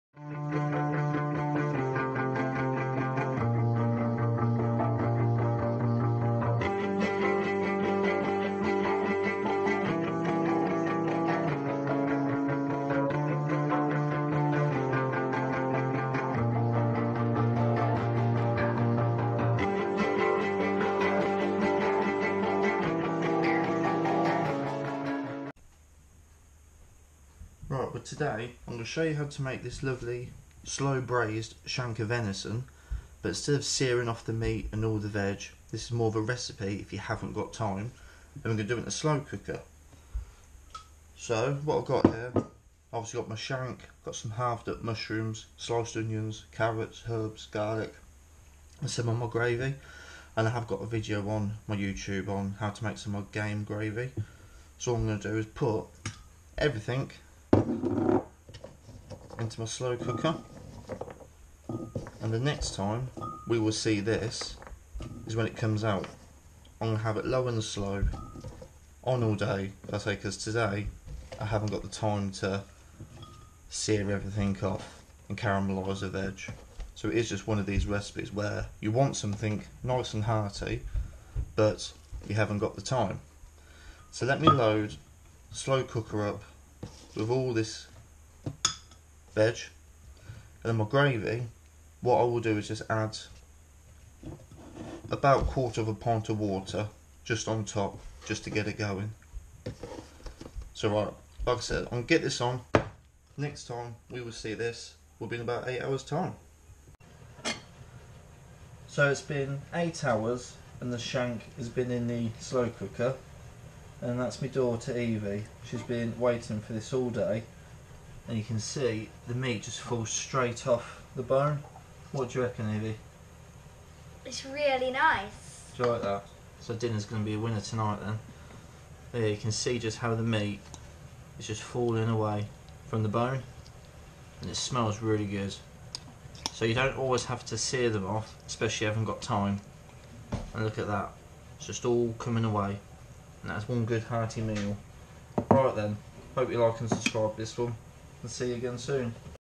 Braised venison shank (the easy way)
Using a fantastic cut of meat that melts in the mouth, this recipe is if you don't have time but want a good meal at the end of the day.